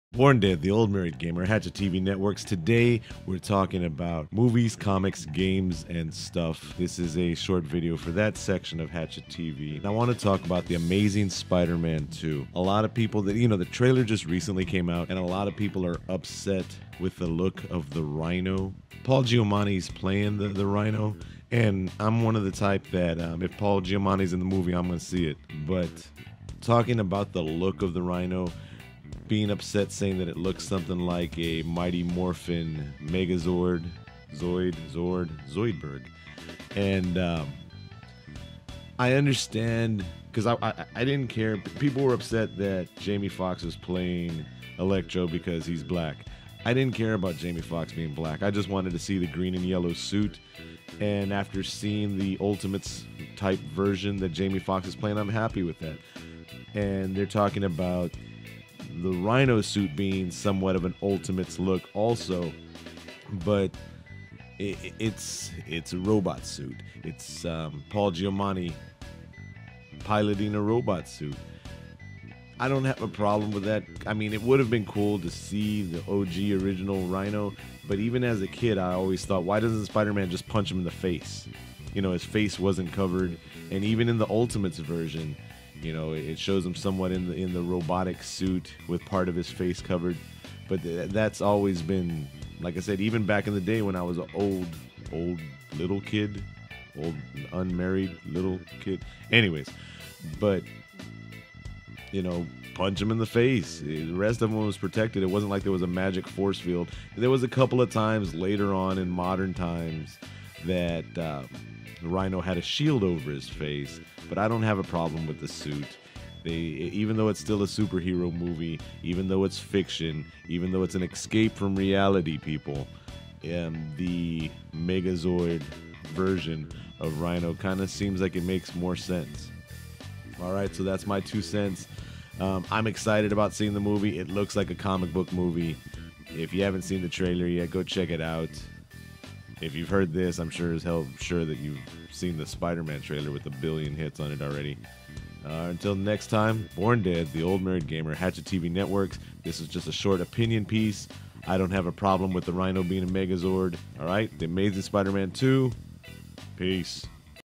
The Rhino-Zord in The Amazing Spider-Man 2? (Movies, Comics, Games, and Stuff Opinion)
The Rhino-Zord in The Amazing Spider-Man 2? Why is everyone so mad about the zord?
This is for the Hatchet TV Movie Comic Games and stuff section,